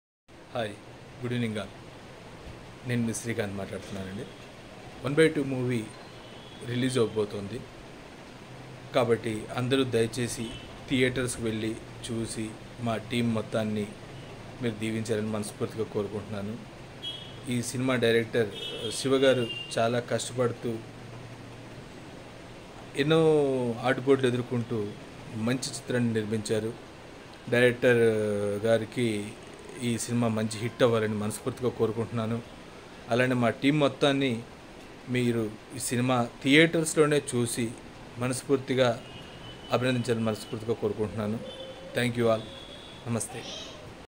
Publicity Designer Srikanth about "One By Two " Movie | Shiva Eturi | Sri Pallavi | DSK Screen
Starring : Sai kumar, Anand Pandi, Sri Pallavi, Devi Prasad, kasi Viswanath & others.
Directed By : Shiva Eturi
Produced By : Karanam Srinivasa Rao
Banner : Cherry Creative Works
Music Director : Leander Lee Marty
Cinematography : Shankar Kesari
Editor : JP
Executive Producer : Janakirama Rao Pamaraju
Dialogues : Vijaya bharati
PRO : Sai Sathish
Stills : Moorthi K
Publicity design : Srikath